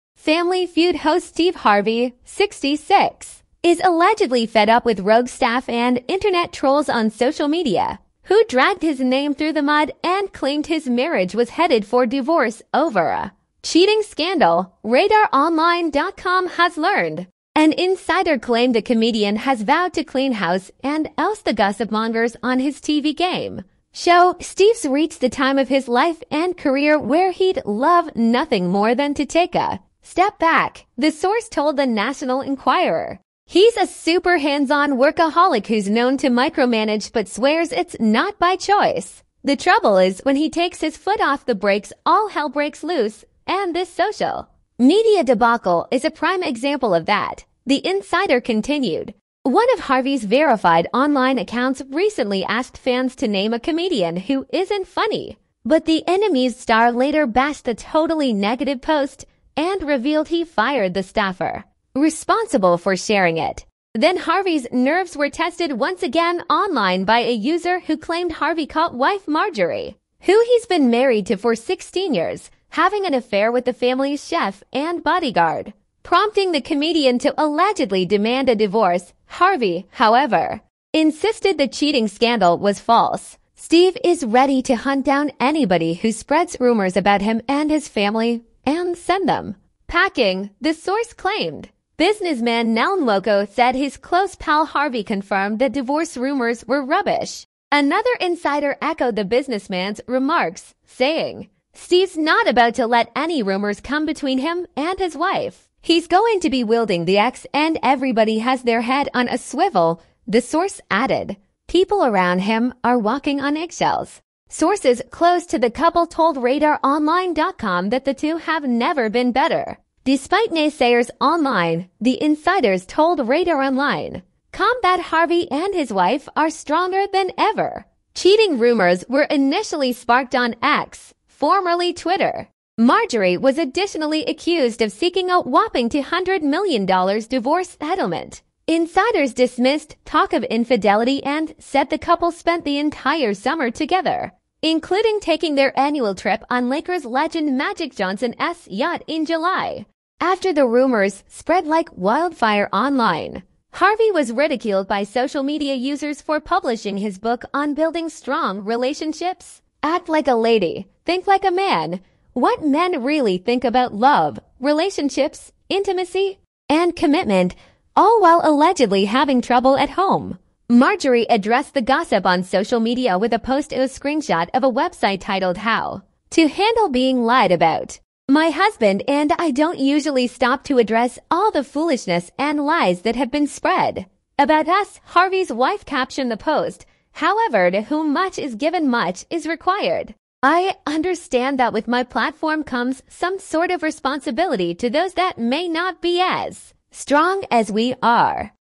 Steve Harvey Takes Action Against Cheating Scandal: Determined to Confront Rumor Spreaders
Steve Harvey Vows to Clean House After Wife Cheating Scandal: 'He's Ready to Hunt Down Anybody Who Spreads Rumors'